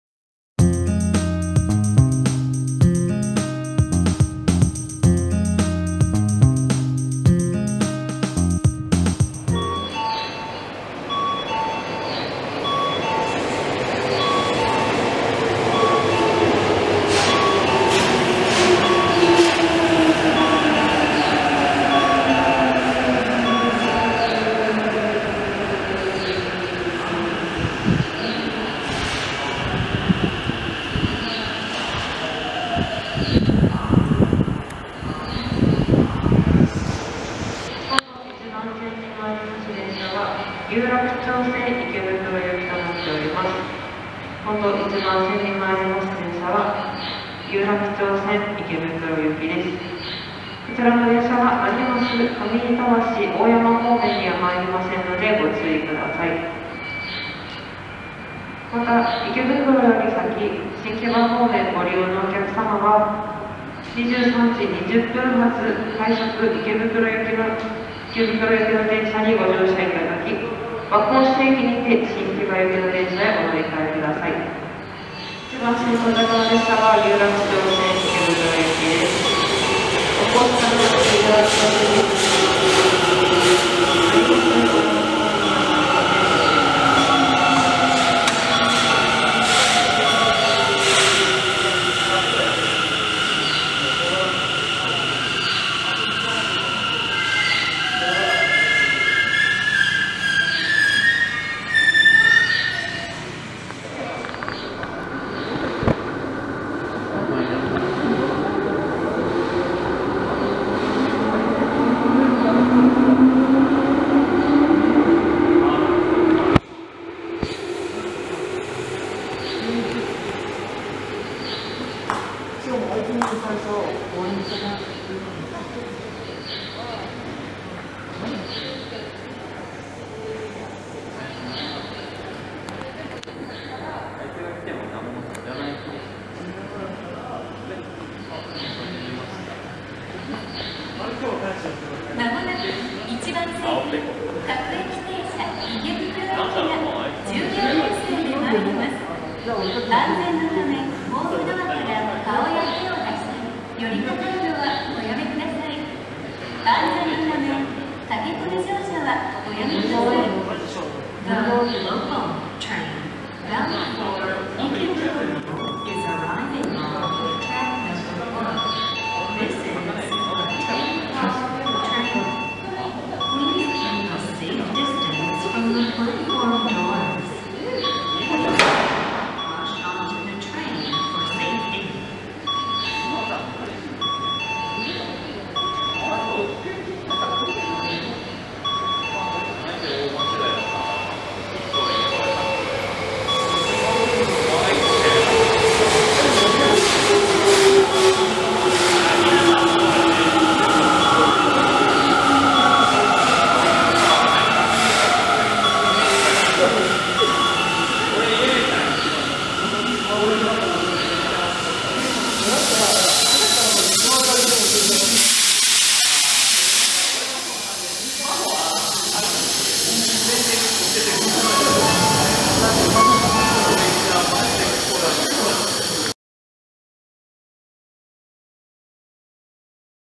東武東上線「２つの池袋行」その後~ダイヤ改正後の表示はどうなった？~
2021年1月20日の終電繰り上げで誕生した東武東上線の「有楽町線 池袋行」。2021年3月13日のダイヤ改正で、晴れて定期列車となりました(平日ダイヤの列車なので、改正後の初列車は3月15日)。
急遽実施された終電繰り上げのときは駅の表示が対応していませんでしたが、その後表示方法を変え、さてダイヤ改正後はどんな表示になったのでしょうか。

ダイヤ改正直前とダイヤ改正後の川越駅の表示の変化をお届けします。